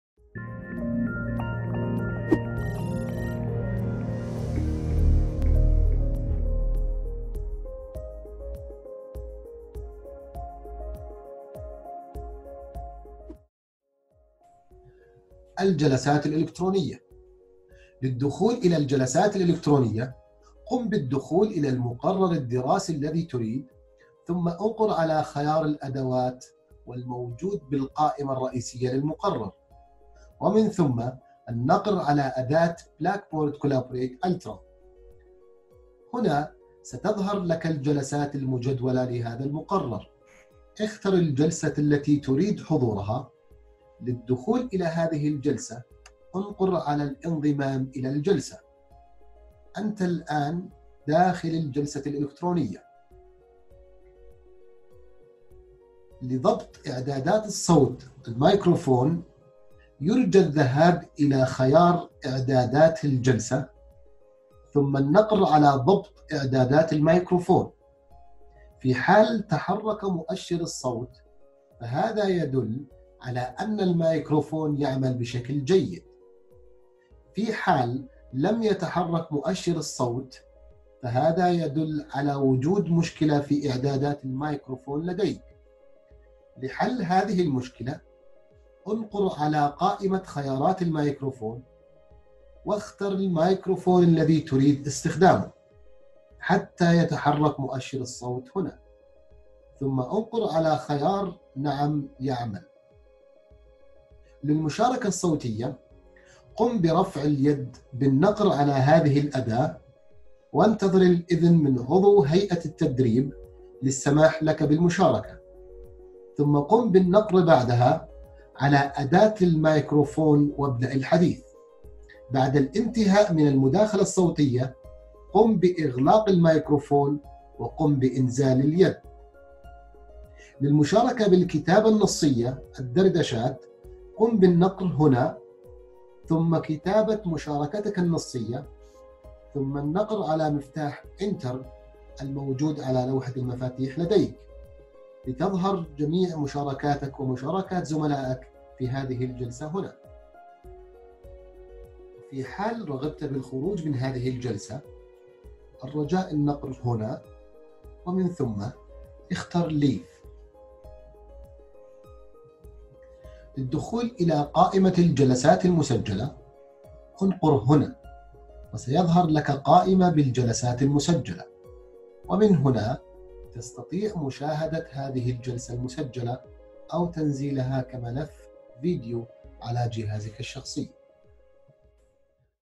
فيديو طريقة الدخول الى الجلسات الإلكترونية للدارسين تعديل
طريقة الدخول الى الجلسات الالكترونية في معهد الادارة العامة